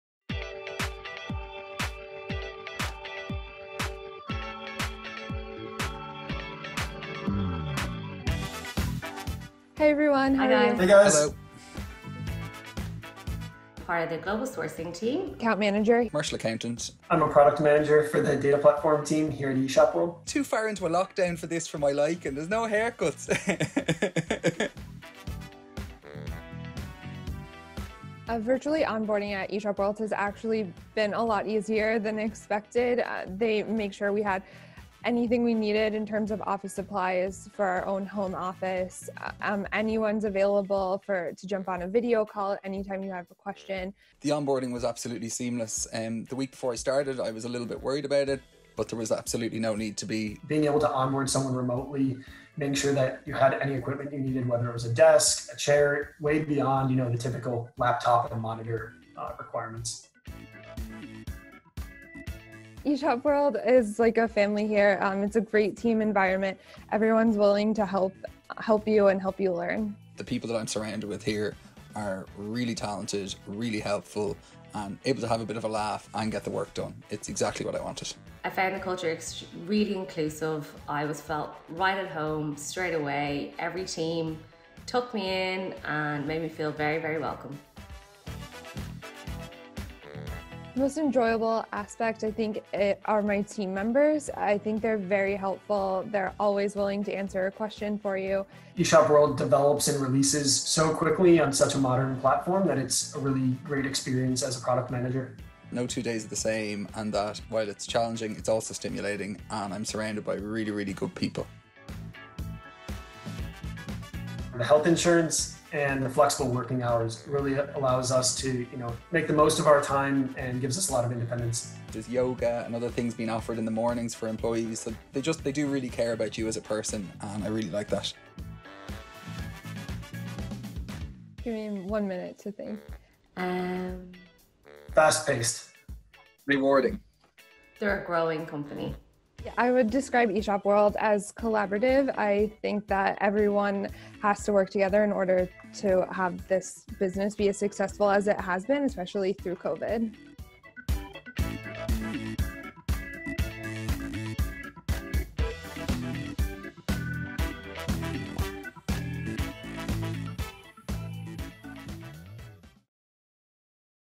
Virtual onboarding at eShopWorld | Jobbio
Over 150 people have joined the eShopWorld global offices since March 2020! We caught up with 4 of new joiners to find out about their journey so far and hear a little about their virtual onboarding experience.